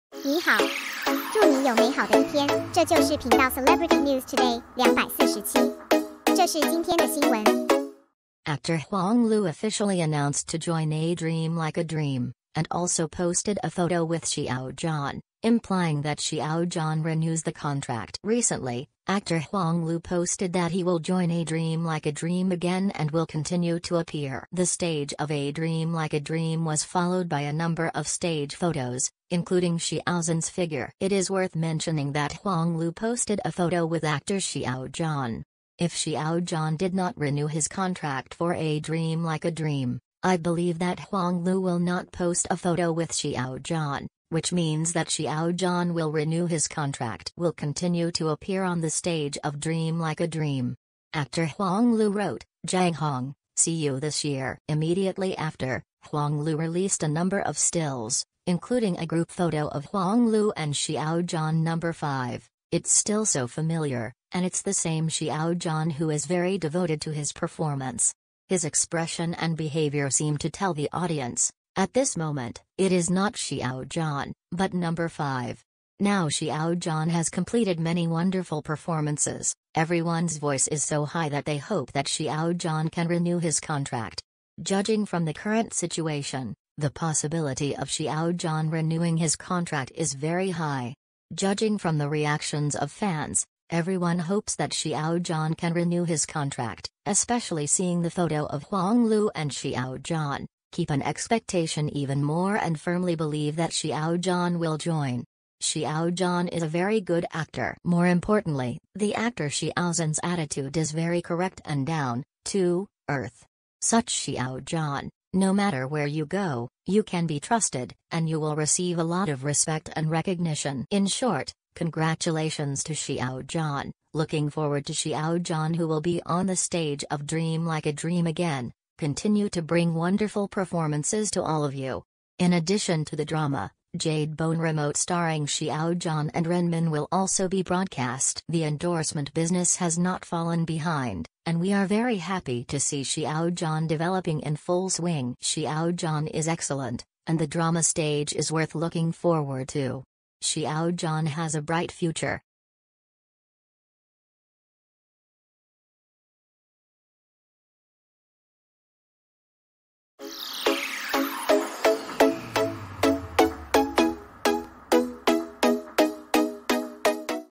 Actor Huang Lu officially announced to join "A Dream Like a Dream", and also posted a photo with Xia
*Celebrity News Today 247 2022/06/19 : Actor Huang Lu officially announced to join "A Dream Like a Dream", and also posted a photo with Xiao Zhan, implying that Xiao Zhan renews the contract ...
 Actor Huang Lu officially announced to join "A Dream Like a Dream", and also posted a photo with Xi